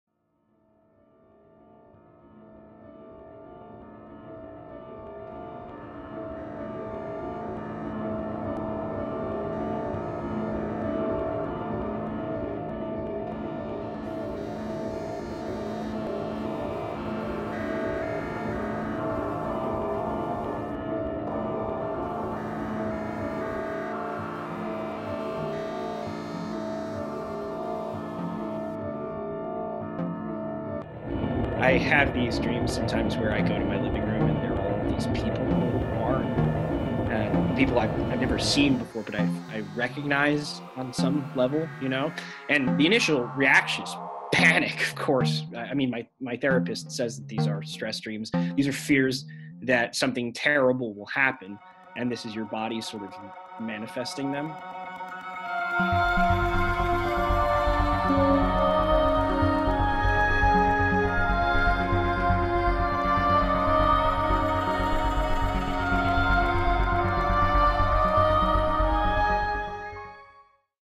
trailer - remote: a new play for zoom - May 22/23
fullscreen blackbox will be hosting two live performances of Joey Santia’s new play for zoom :: remote. MAY 22//23 @8:30pm EST
All ticket donations will be towards the benefit of the Stonewall Community Foundation, an organization dedicated to offering assistance and support to the communities in New York City made most vulnerable by pandemic.
Set in the year 2059, remote is a play told in eight vignettes that explores a world that is totally dependent on Zoom, creating the conditions for an Amazon global monopoly, pay-for-friendship services, and an annual tradition of E-Lympics. But underneath the grayness of isolation, something more sinister is taking shape, as the connective tissue of a stranded world stretches to its breaking point. remote will be performed live in a zoom webinar. Ticket-holders will be notified of access to the webinar link 30 minutes prior to curtain.
dir. talley murphy pr. rose meriam & sophia seidenberg a fullscreen blackbox production _noesc